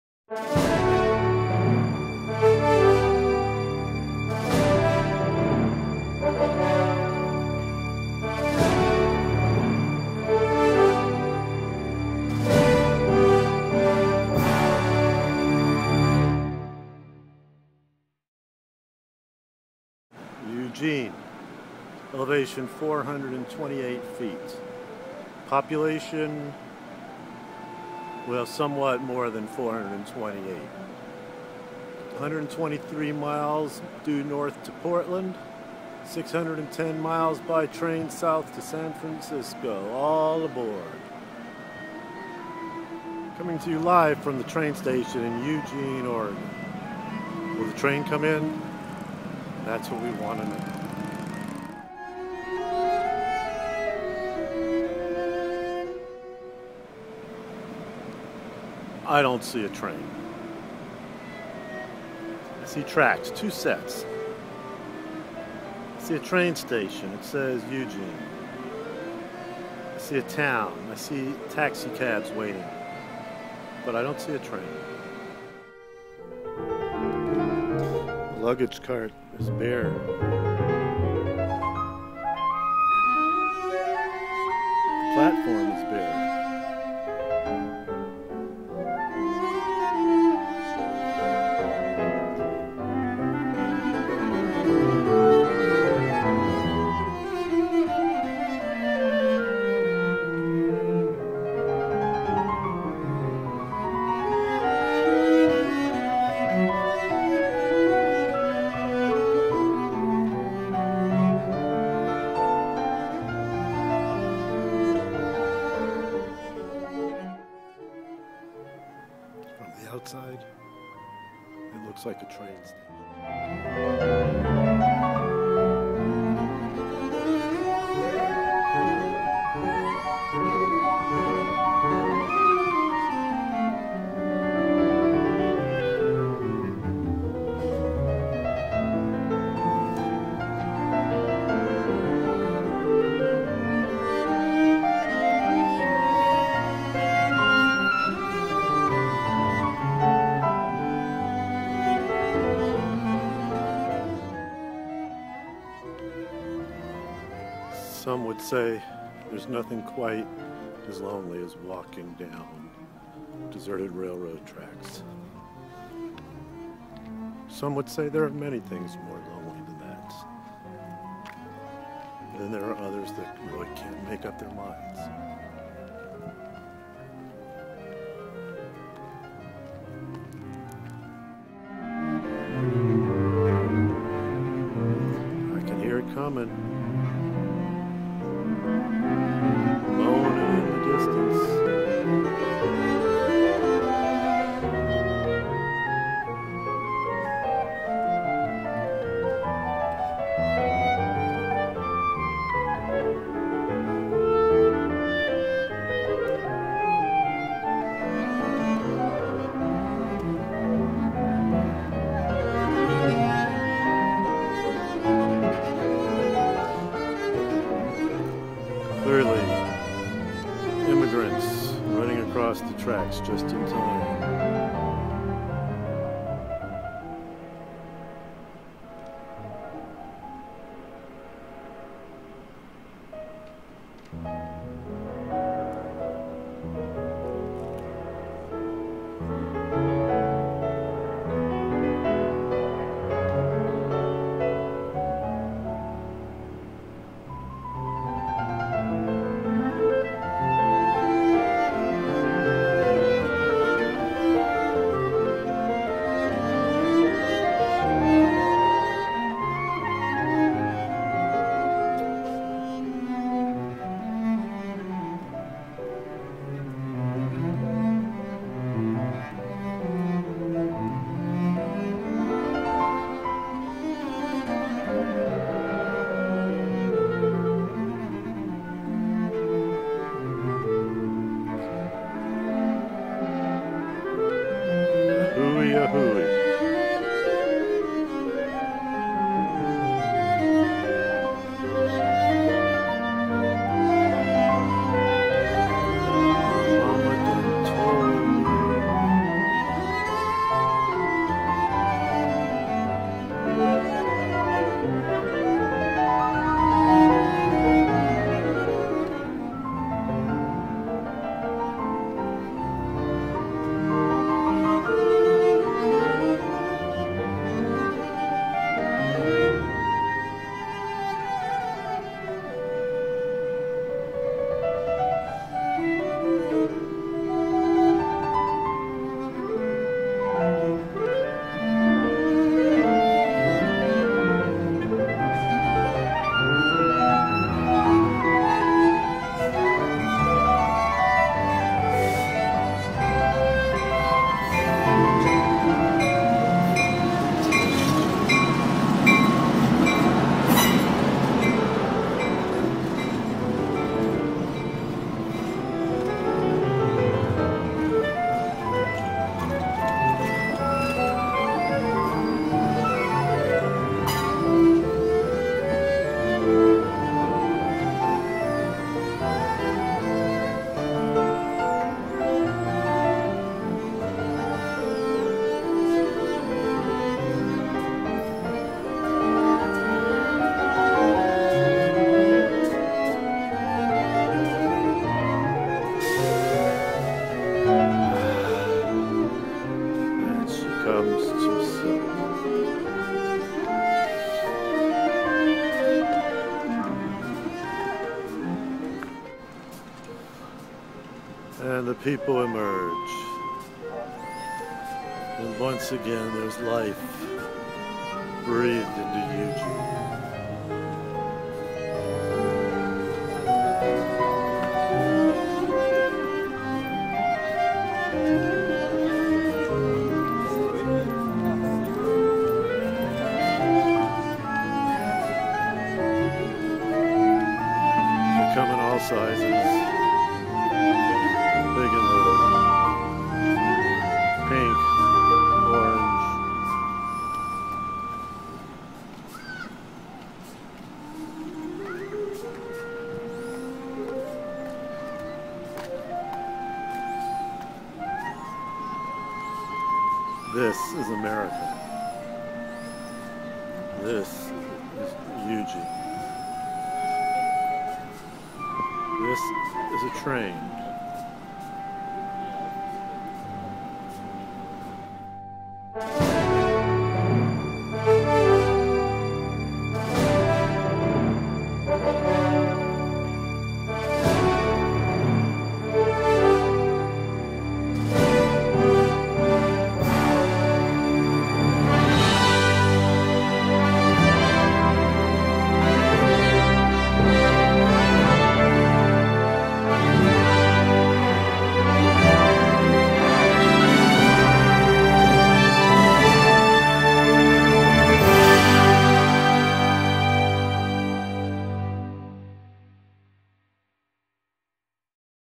On the Road - Eugene Train Depot
A look at the Eugene Train Depot.

Music performed by Ricardo Morales, Jesus Morales and David Riley, Composed by Terry Vosbein.

Part of the ongoing series ON THE ROAD: The madcap adventures of a boy, his monkey and the open road.